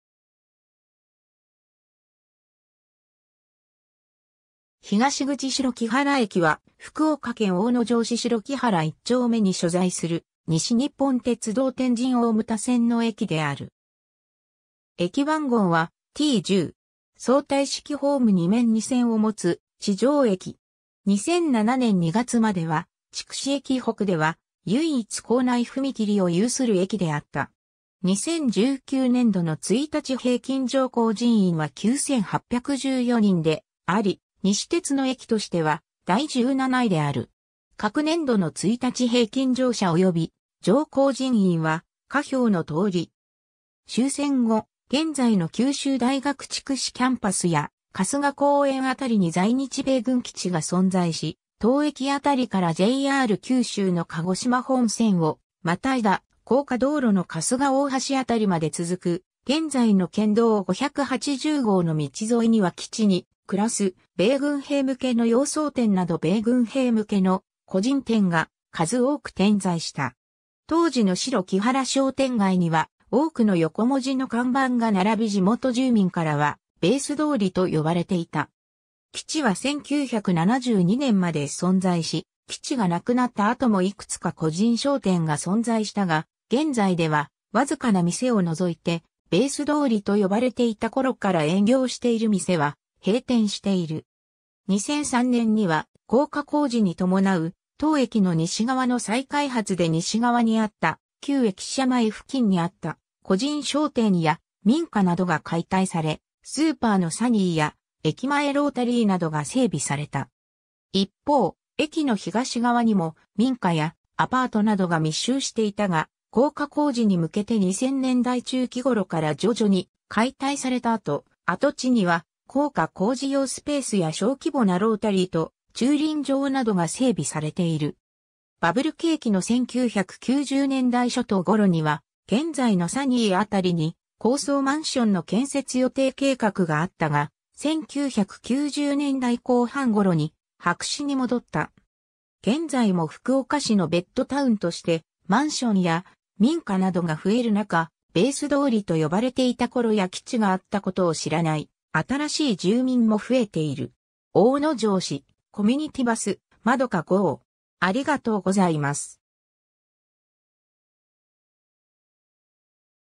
白木原駅
東口 白木原駅（しらきばるえき）は、福岡県大野城市白木原一丁目に所在する西日本鉄道（西鉄）天神大牟田線の駅である。
 相対式ホーム2面2線を持つ地上駅。
2007年2月までは、筑紫駅以北では唯一構内踏切を有する駅であった。
 2019年度の1日平均乗降人員は9,814人 であり、西鉄の駅としては第17位である。
 各年度の1日平均乗車および乗降人員は下表のとおり。
 終戦後、現在の九州大学筑紫キャンパスや春日公園辺りに在日米軍基地が存在し、当駅辺りからJR九州の鹿児島本線を跨いだ高架道路の春日大橋辺り(当時は基地に入るゲートがあった)まで続く現在の県道580号の道沿いには基地に暮らす米軍兵向けの洋装店など米軍兵向けの個人店が数多く点在した。
当時の白木原商店街には多くの横文字の看板が並び地元住民からは「ベース通り」と呼ばれていた。
基地は1972年まで存在し、基地が無くなった後もいくつか個人商店が存在したが、現在では僅かな店を除いて「ベース通り」と呼ばれていた頃から営業している店は閉店している。
2003年には、高架工事に伴う当駅の西側の再開発で西側にあった旧駅舎前付近にあった個人商店や民家などが解体されスーパーのサニーや駅前ロータリーなどが整備された。
一方、駅の東側にも民家やアパートなどが密集していたが、高架工事に向けて2000年代中期頃から徐々に解体された後、跡地には高架工事用スペースや小規模なロータリーと駐輪場などが整備されている。
バブル景気の1990年代初頭頃には、現在のサニー辺りに高層マンションの建設予定計画があったが、1990年代後半頃に白紙に戻った。
現在も福岡市のベッドタウンとしてマンションや民家などが増える中、ベース通りと呼ばれていた頃や基地があったことを知らない新しい住民も増えている。
 大野城市コミュニティバス「まどか号」